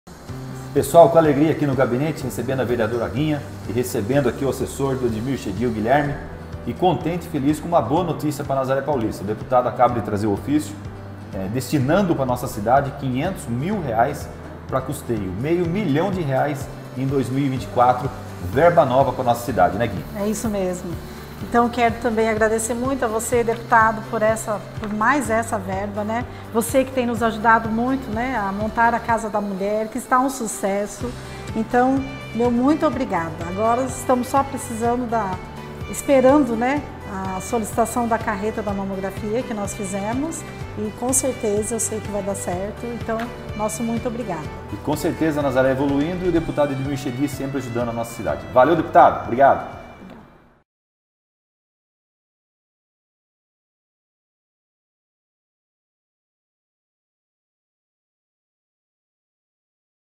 DEPUTADO EDMIR CHEDID GARANTE R$500MIL PARA CUSTEIO DE NAZARÉ PAULISTA
“Através da solicitação da vereadora Guinha Canêdo”

O prefeito Murilo Pinheiro recebeu nesta segunda-feira(26) em seu gabinete, ofício da Casa Civil do Governo do Estado de São Paulo confirmando a emenda parlamentar do deputado estadual Edmir Chedid, através da solicitação da vereadora Guinha Canêdo, no valor de R$ 500 mil para custeio da área de Saúde.

O prefeito municipal agradece ao deputado estadual Edmir Chedid pela Emenda Parlamentar. “Agradeço o deputado Edmir Chedid pela Emenda Parlamentar, e a vereadora Guinha Canêdo por sempre buscar recursos para nossa cidade”, enfatizou o prefeito.

Estiveram presentes o assessor parlamentar Guilherme Gorsky e a vereadora Guinha Canêdo.